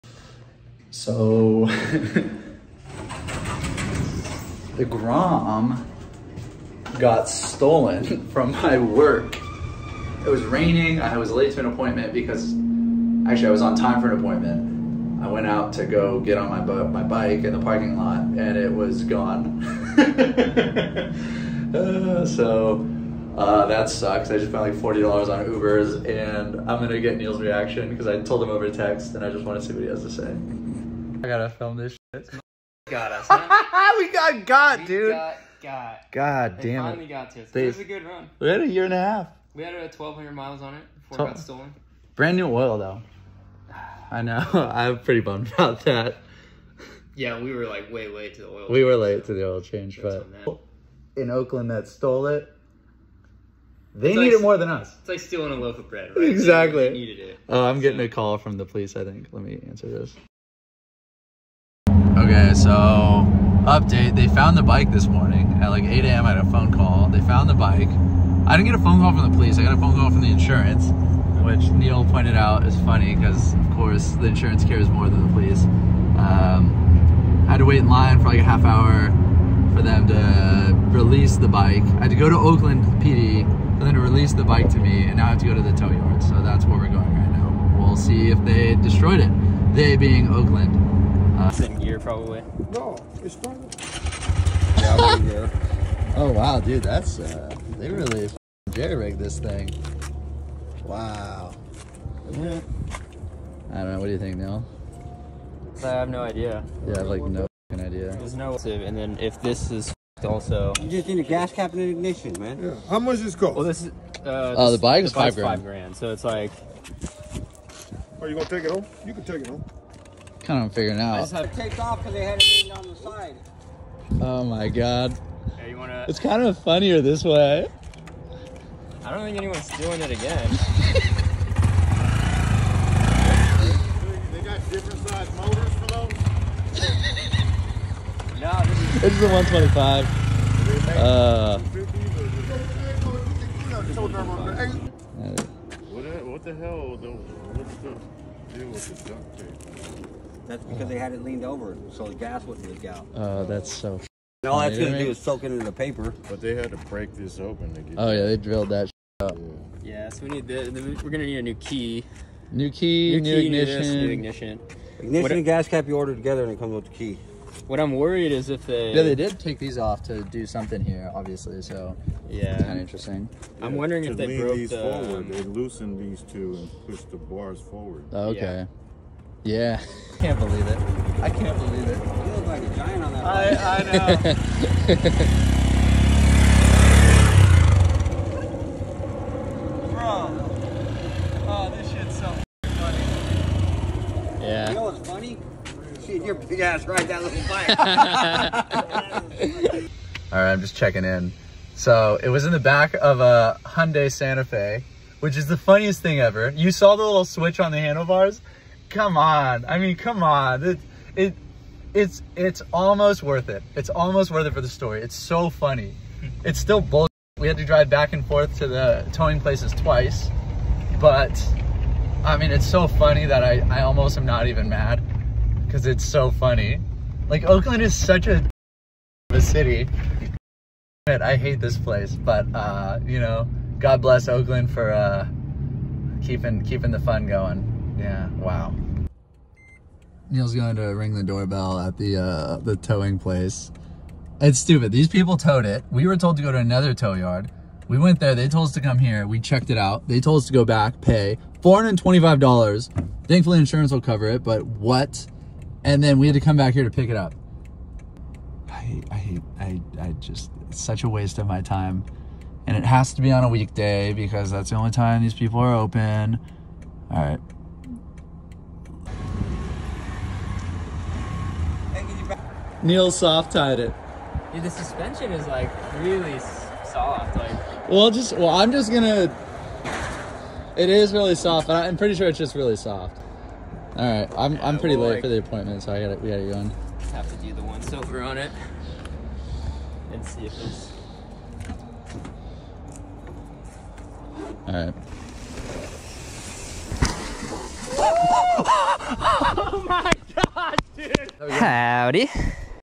Recovering Our STOLEN HONDA GROM FROM OAKLAND!
Yeah, so this was about as ridiculous as you can imagine.

The Grom was stolen in the middle of the day from my office parking lot in a nice neighborhood in San Francisco. The police found it in the back of another stolen thing (a car), and we had to spend a Friday afternoon recovering it and driving all over Oakland.

Absolute nonsense.